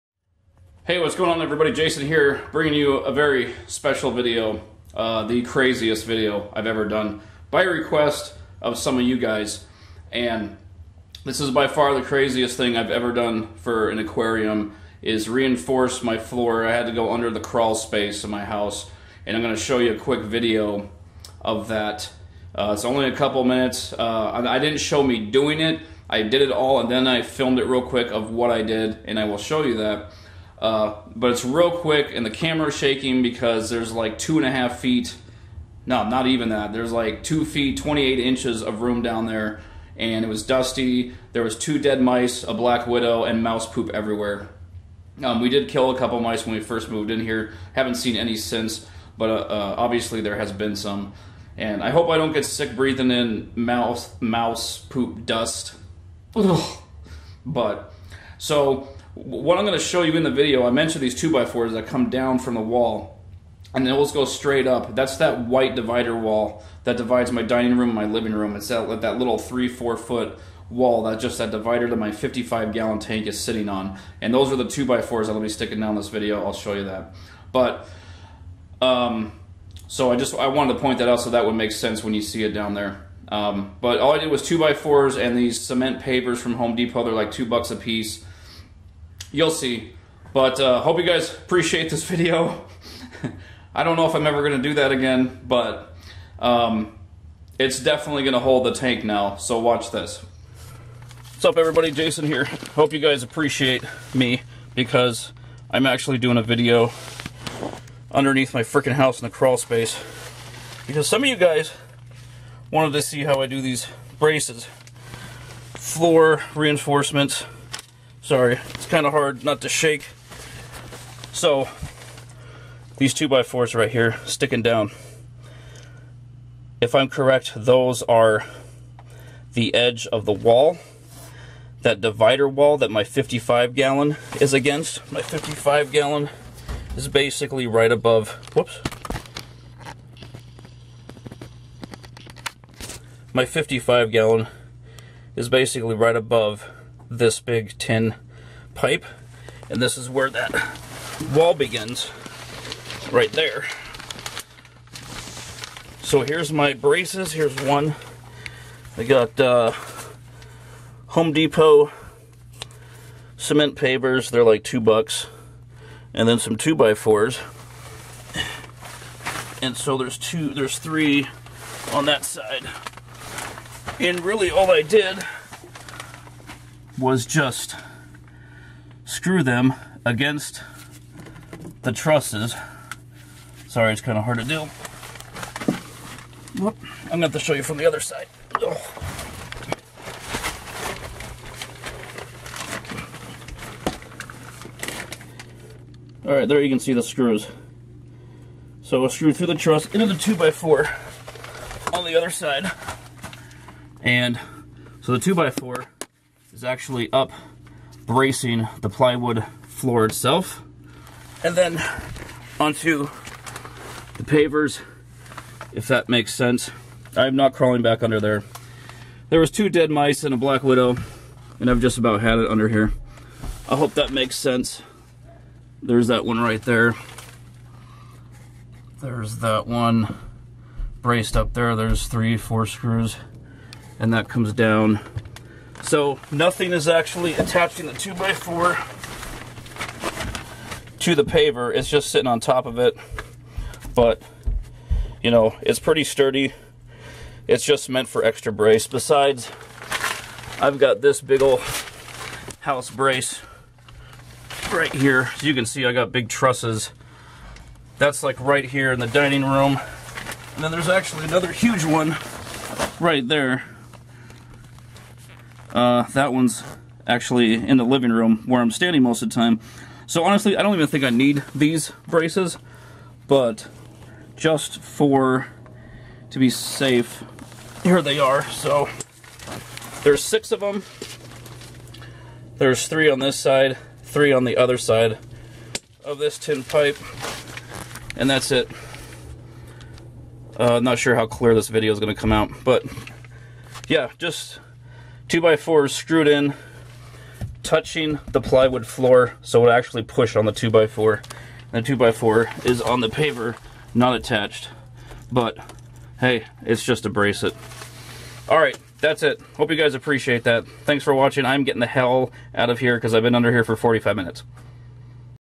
100 GALLON AQUARIUM BUILD PART 6! Reinforcing the living room floor.
A few people requested a video on this so here it is. Just a quick look at what I did in the crawlspace of my house.